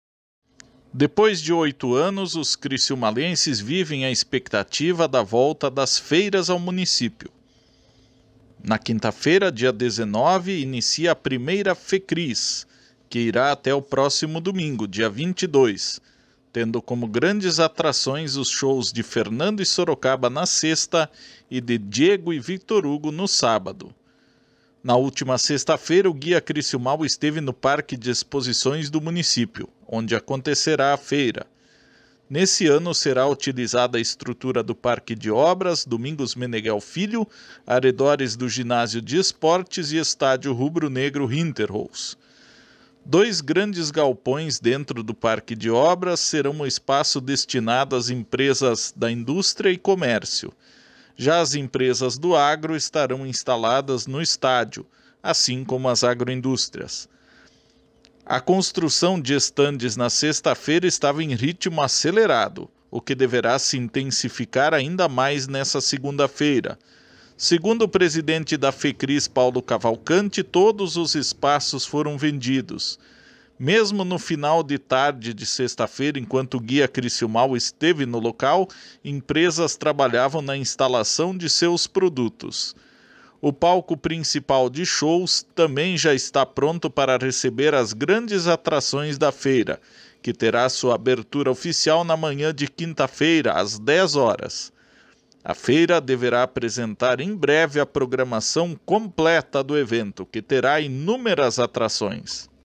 Estandes da FECRIS em ritmo acelerado de montagem
Depois de 08 anos, os crissiumalenses vivem a expectativa da volta das feiras ao município.
Na quinta-feira (19) inicia a primeira FECRIS, que irá até o próximo domingo (22), tendo como grandes atrações os shows de Fernando e Sorocaba na sexta e de Diego e Victor Hugo no sábado.
Na última sexta-feira o Guia Crissiumal esteve no Parque de Exposições do Município, onde acontecerá a feira. Nesse ano será utilizada a estrutura do Parque de Obras Domingos Meneghel Filho, arredores do Ginásio de Esportes e Estádio Rubro-Negro Hinterholz. Dois grandes galpões dentro do Parque de Obras serão o espaço destinado as empresas de Indústria e Comércio, já as empresas do agro estarão instaladas no estádio, assim como as agroindústrias.
A construção de estandes na sexta-feira estava em ritmo acelerado, o que deverá se intensificar ainda mais nessa segunda-feira. Segundo o presidente da FECRIS Paulo Cavalcanti, todos os espaços foram vendidos. Mesmo no final da tarde, enquanto o Guia Crissiumal esteve no local empresas trabalhavam na instalação de seus produtos.
O palco principal de shows também já está pronto para receber as grandes atrações da feira, que terá a sua abertura oficial na manhã de quinta-feira, às 10h00min.
A feira também deverá apresentar em breve a programação completa, que terá inúmeras atrações.